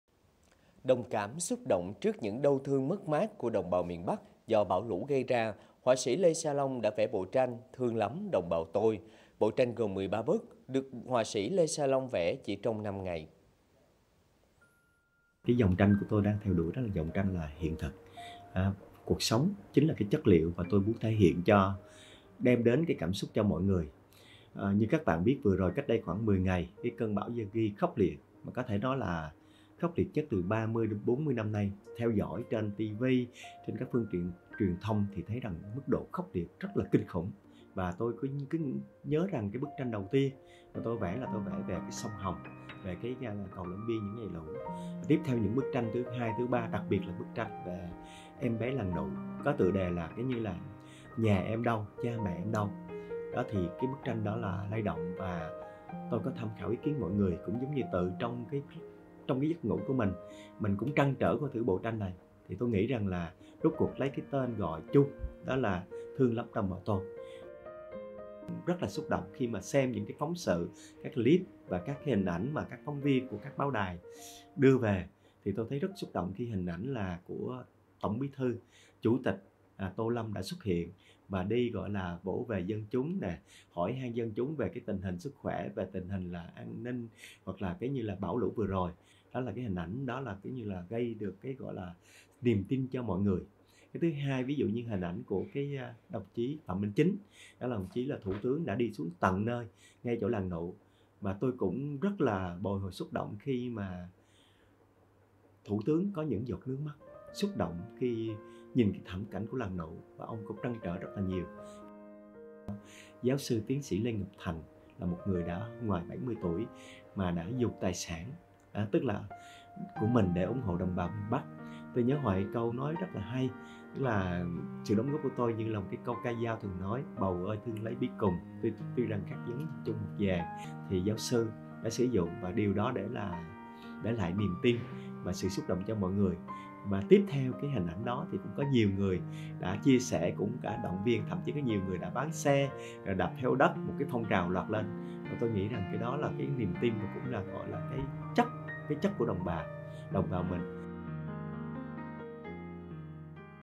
TÌNH NGƯỜI TRONG BÃO LŨ QUA TRANH CỦA LÊ SA LONG
Đồng cảm, xúc động trước những đau thương, mất mát của đồng bào miền Bắc do bão lũ gây ra, họa sĩ Lê Sa Long vẽ bộ tranh "Thương lắm, đồng bào tôi". Bộ tranh gồm 13 bức, được họa sĩ Lê Sa Long vẽ trong năm ngày.